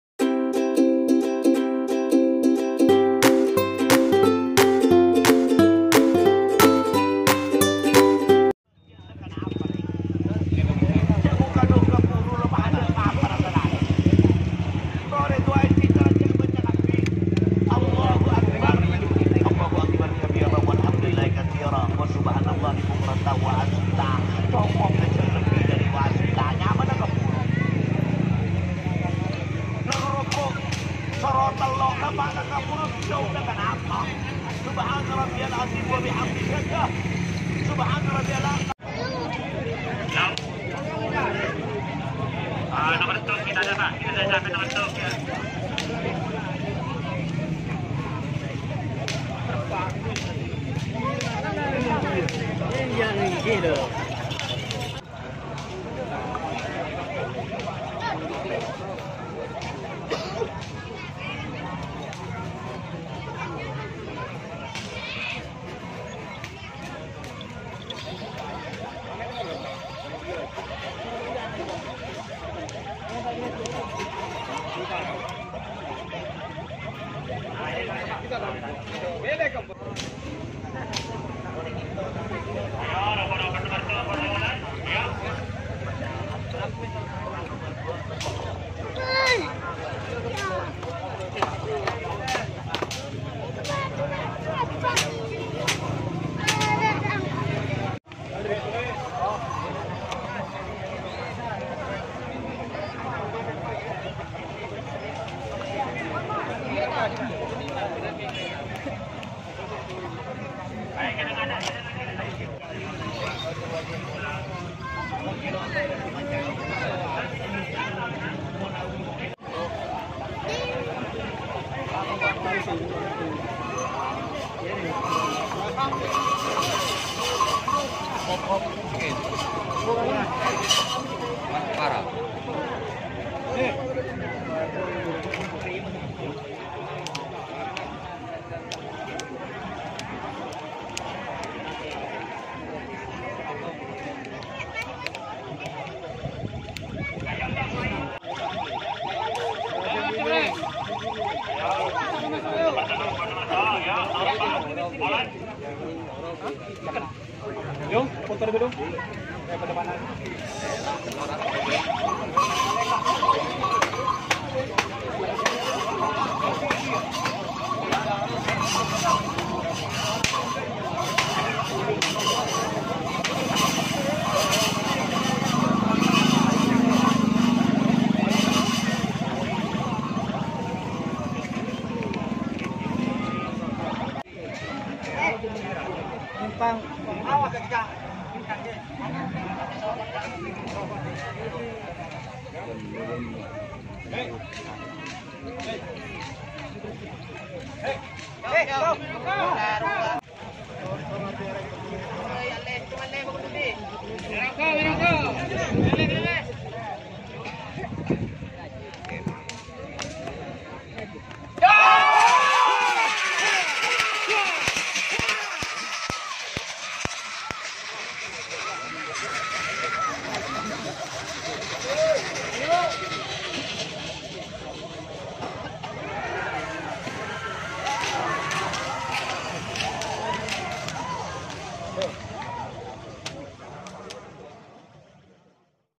Kerapan Sapi//Big Boss Kerap Torjun Sampang
Perhelatan kerapan sapi di Torjun Sampang pembukaan tahun 2020 stat Big Boss Jet Matik Foundetion.